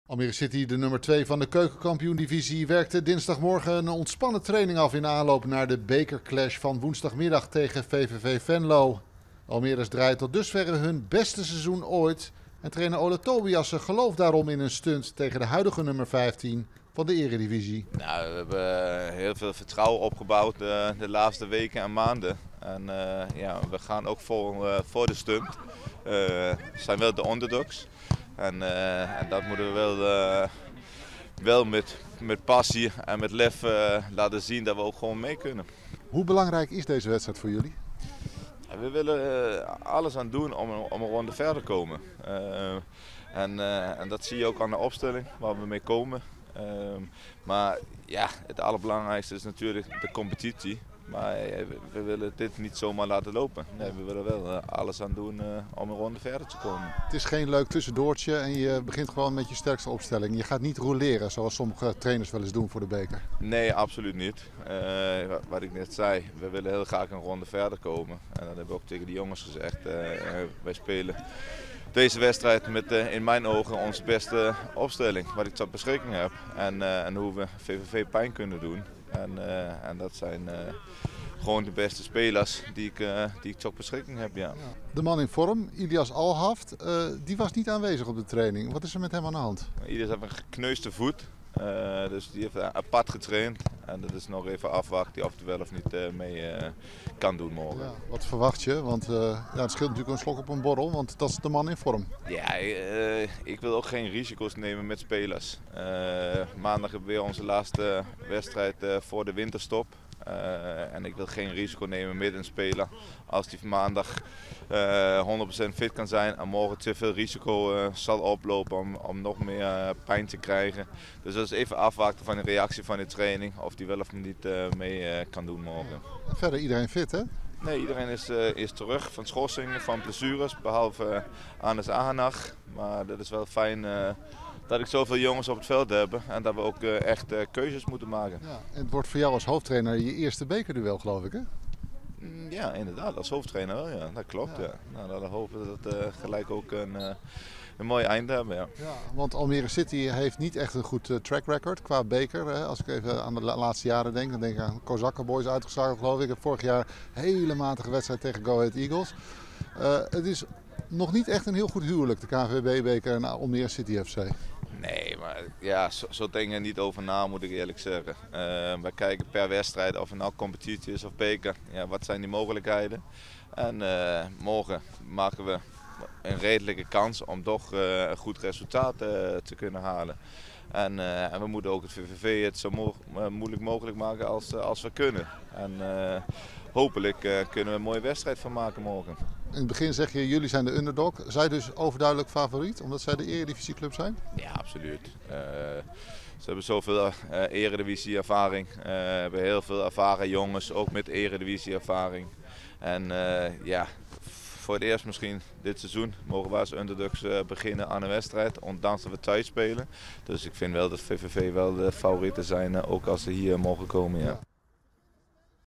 City FC gelooft in bekerstunt tegen VVV Venlo
Almere City FC wil woensdag ten koste van VVV Venlo de derde ronde van het toernooi om de KNVB beker bereiken. De Almeerders nemen het vanaf 16:30 uur in het eigen Yanmar stadion op tegen de huidige nummer 15 van de eredivisie.

Trainer Ole Tobiasen kan tegen de Limburgers beschikken over een fitte selectie. Alleen aanvaller Ilias Alhaft is een serieus twijfelgeval. Alhaft heeft een gekneusde voet overgehouden aan de uitwedstrijd tegen Telstar van afgelopen vrijdag en trainde niet mee met de selectie dinsdagmorgen.

In het verleden troffen Almere City FC en VVV Venlo elkaar voor de competitie al zeven keer in Almere (toen VVV nog in de eerste divisie speelde). Nog nooit wisten de Almeerders te winnen van de Limburgers: vijf keer werd er verloren en twee keer werd er gelijkgespeeld.

De bekerwedstrijd in Almere staat woensdag onder leiding van scheidsrechter Bas Nijhuis. Bij VVV Venlo speelt aanvaller Torino Hunte, die vorig seizoen nog voor Almere City FC speelde, maar niet kon rekenen op contractverlenging.